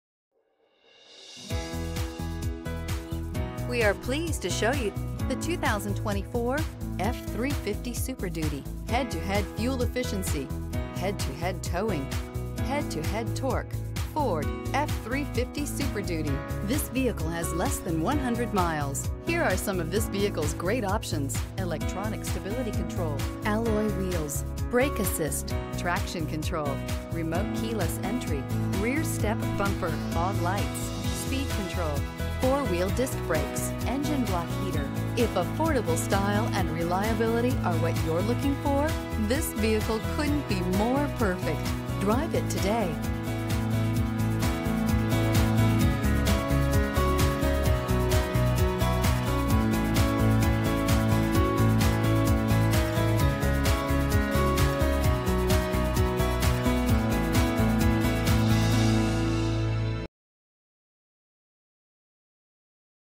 2024 Ford F-350SD 24021
2024 Ford F-350SD XLT

**AVAILABLE FOR IMMEDIATE DELIVERY**6.7L POWER STROKE DEISEL**BLACK APPEARANCE PACKAGE**LED BOX LIGHTING**360 DEGREE CAMERA**360 DEGREE CAMERA**HEATED SEATS**5TH WHEEL PREP PACKAGE**SNOW PLOW PREP PACKAGE**ROOF CLEARANCE LIGHTS**PRO POWER ONBOARD**REMOTE START**MUCH MUCH MORE**Agate Black Metallic 2024 Ford F-350SD XLT BLACK APPEARANCE PACKAGE 4WD 10-Speed Automatic Power Stroke 6.7L V8 DI 32V OHV Turbodiesel *NAVIGATION, *FULLY SERVICED, *REMOTE START, *REARVIEW CAMERA, *TRAILER TOW PACKAGE, *HEATED SEATS, 4WD, Navigation System.brbrRecent Arrival!